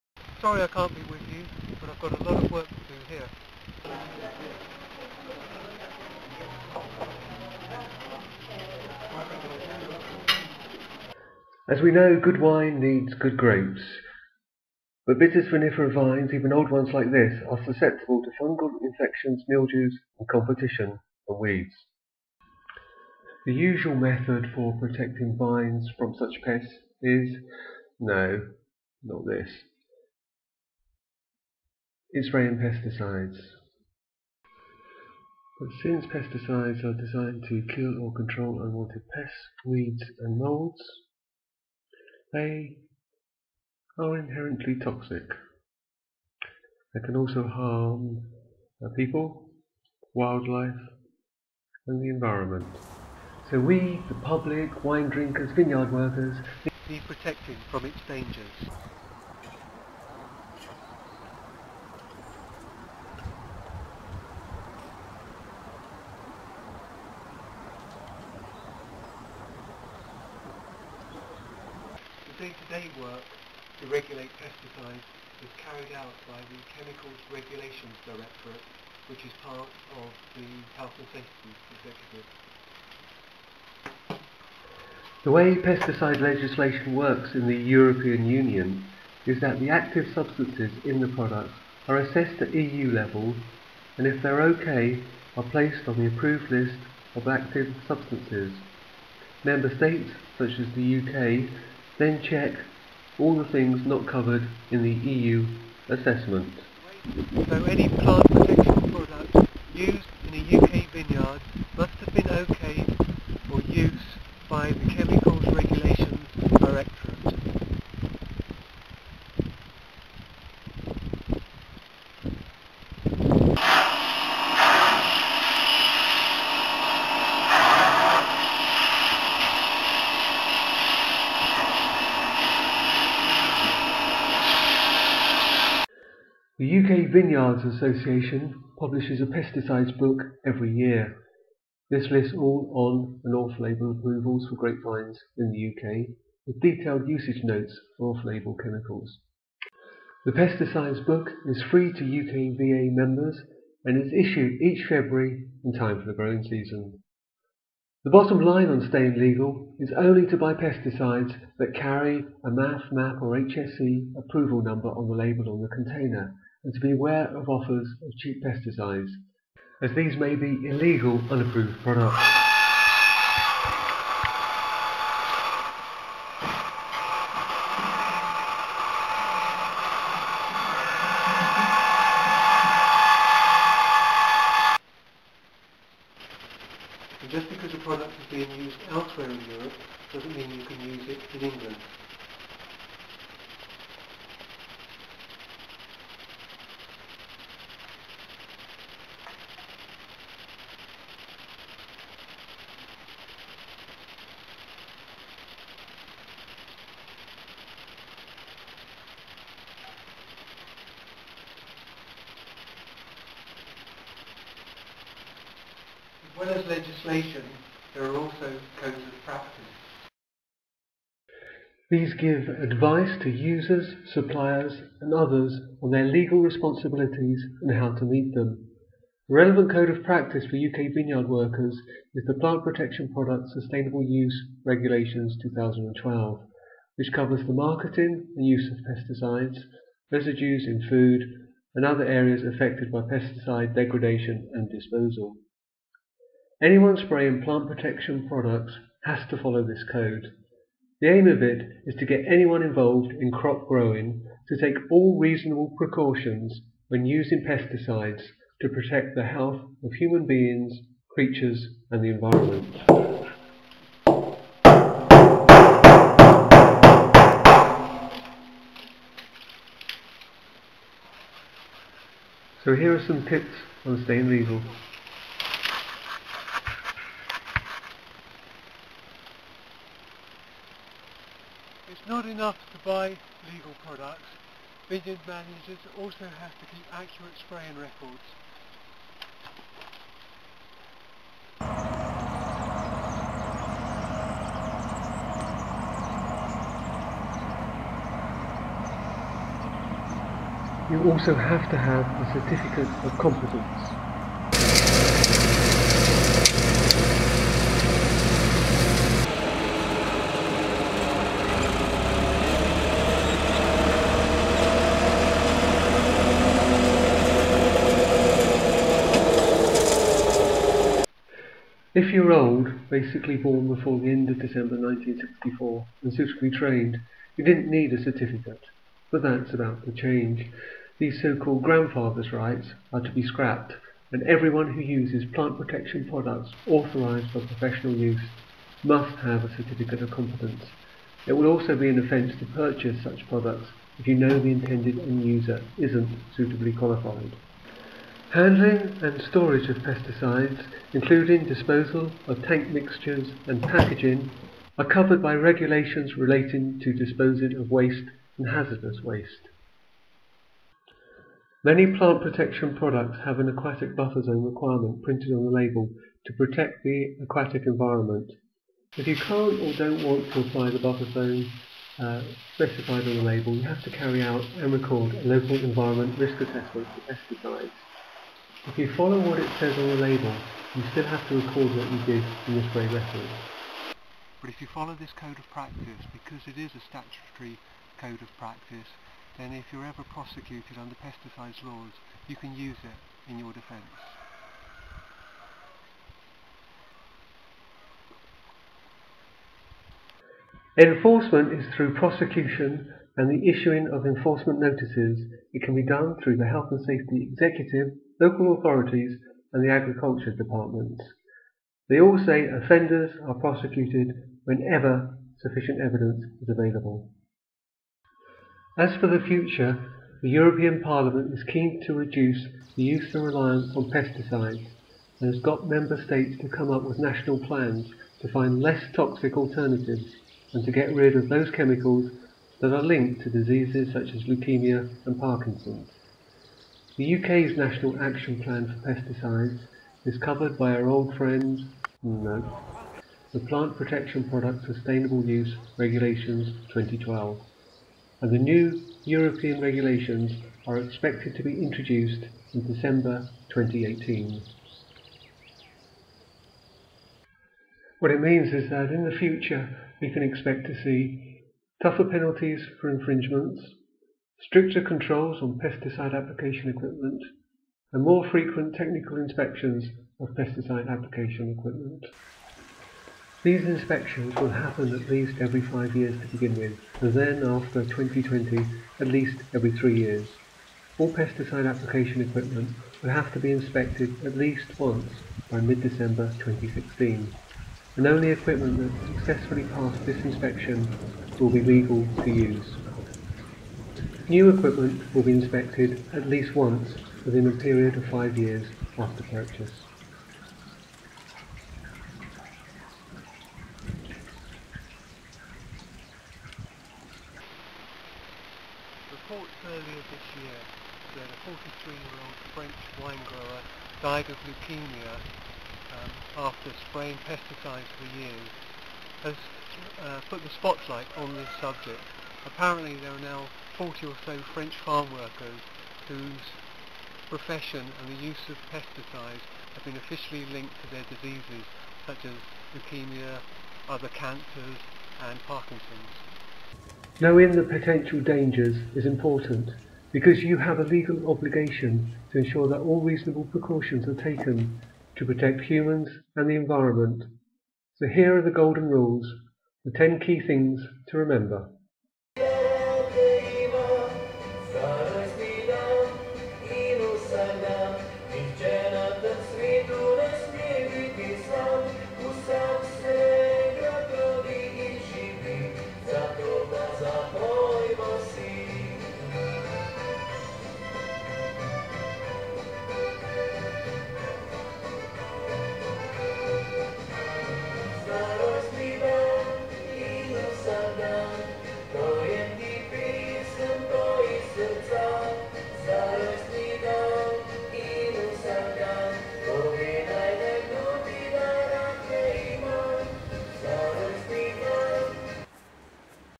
Pesticides legislation video 2
An introduction to pesticides legislation for vine pathology students at Plumpton College